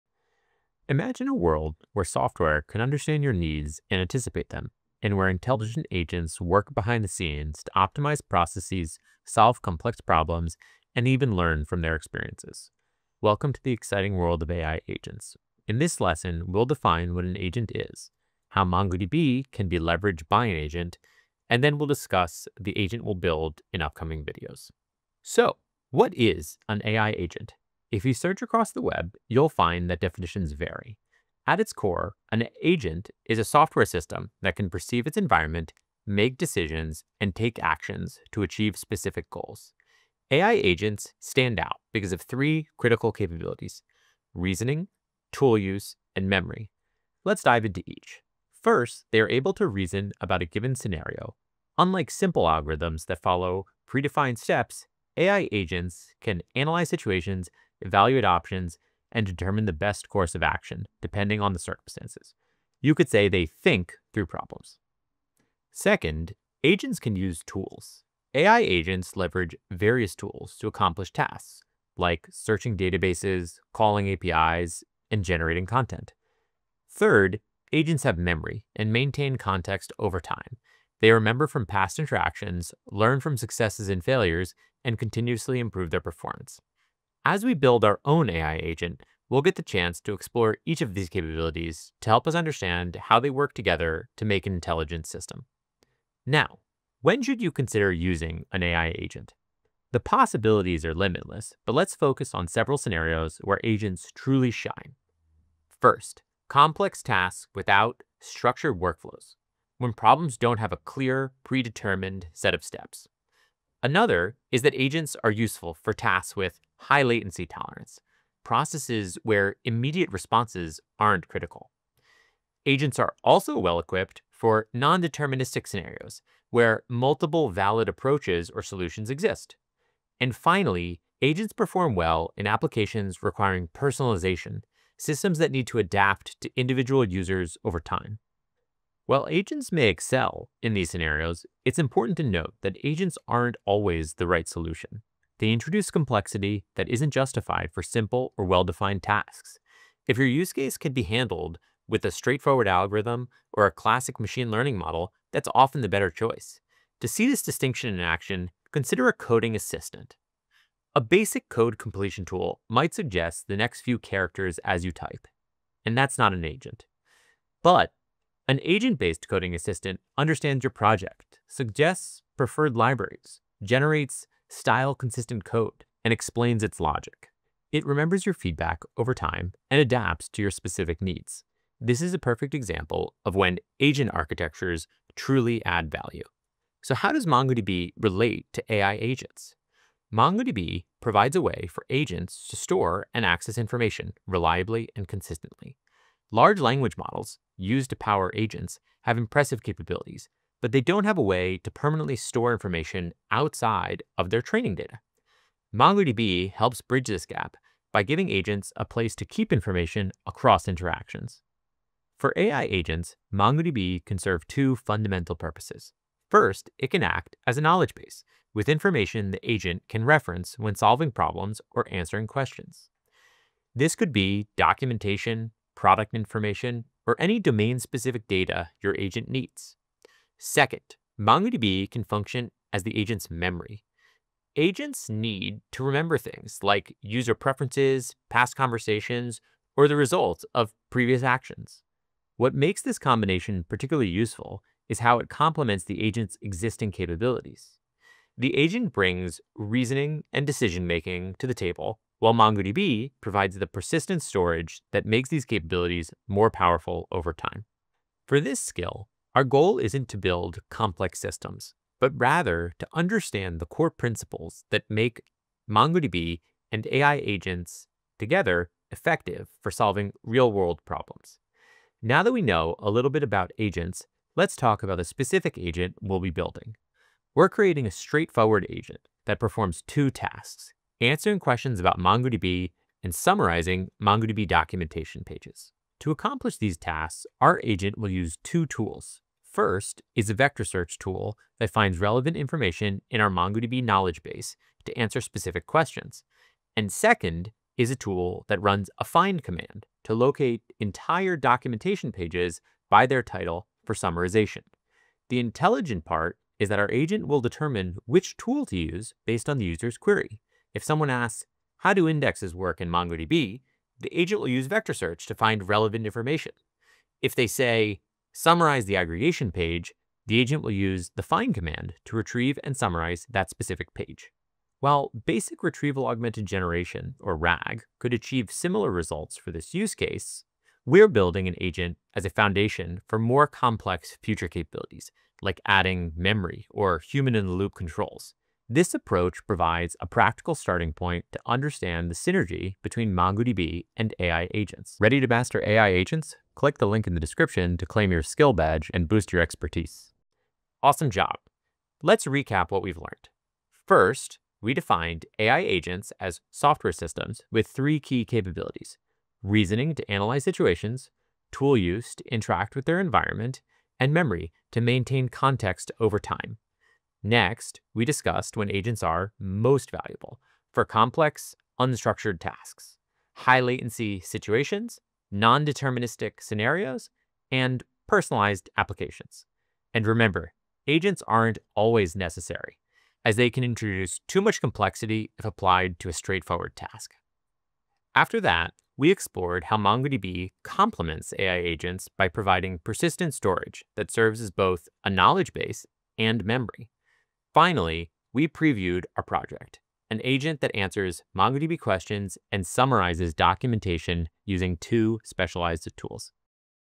What are AI Agents? Reasoning, Tool Use, and Memory Explained | MongoDB AI Fundamentals
Learn more about AI Agents and earn a free Skills Badge

Explore the exciting world of AI agents and learn how they can optimize processes, solve complex problems, and learn from experience. This video defines AI agents, discusses their core capabilities (reasoning, tool use, and memory), and explains when to use them. Discover how MongoDB can be leveraged as a knowledge base and memory for AI agents, providing persistent storage for enhanced capabilities. We'll also preview building an AI agent that answers MongoDB questions and summarizes documentation.

Chapters:
0:00 - Introduction to AI Agents
0:26 - What is an AI Agent?
0:40 - Key Capabilities of AI Agents (Reasoning, Tool Use, Memory)
1:37 - When to Use AI Agents
2:19 - When NOT to Use AI Agents
2:37 - AI Agent vs. Basic Code Completion Example
3:07 - MongoDB's Role with AI Agents
3:30 - MongoDB as Knowledge Base and Memory
4:19 - Overview of the Agent Being Built
4:46 - Tools Used by the Agent
5:06 - How the Agent Determines Tool Use
5:26 - Why Build an Agent for This Use Case
5:48 - Recap